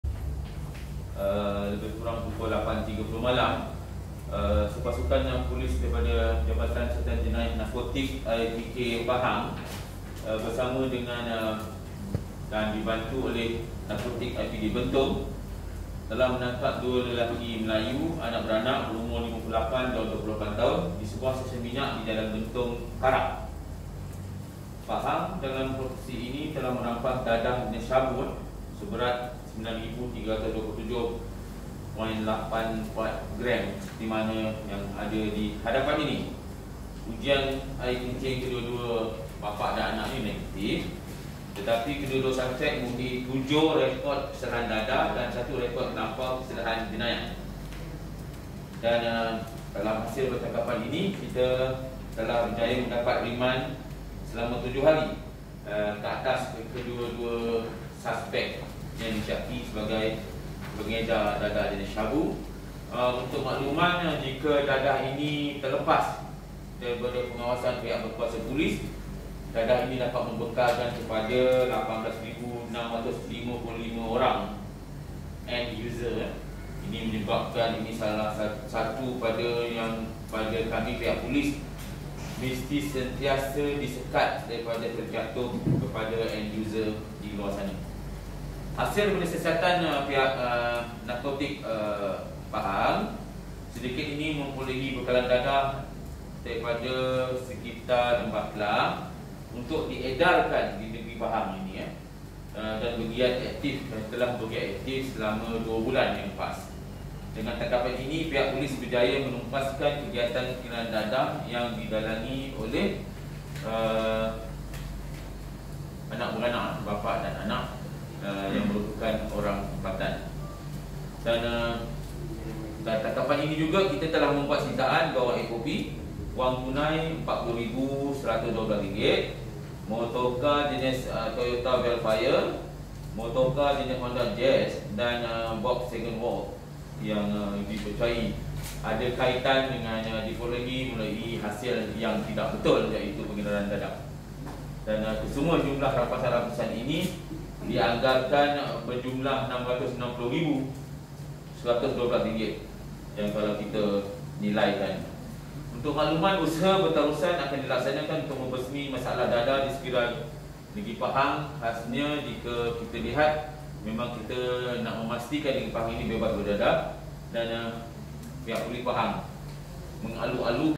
卖毒品致富父子落网 警方起获36万毒品
（关丹25日讯）转行“卖毒品”致富，嗜毒前科父子落网，总值逾66万令吉毒品、现金与房车遭充公。

一对分别是28岁及58岁的巫裔父子于本月21日晚上8时30分，在文冬加叻某油站范围，遭闻讯而至的肃毒警队围捕，双双落网。

警方从父子乘坐的国产华嘉轿车，搜获利用茶叶铝铂袋包装的冰毒，重约9327.84克，估计可“满足”至少1万8655名嗜毒者所需分量。

（本报陈楸妤摄）